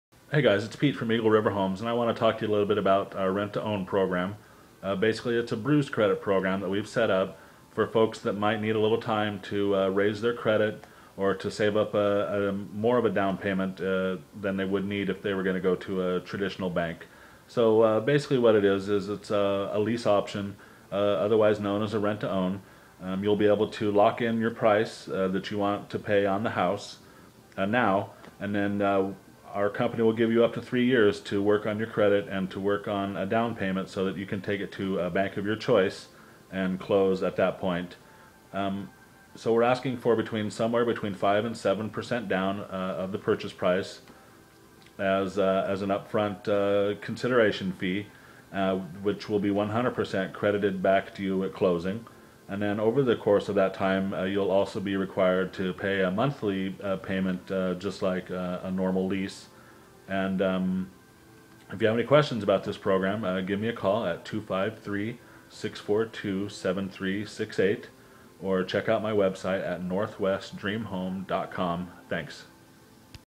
Own your home with a Lease Option agreement
Find out how you can buy a home now with little down and proof of income.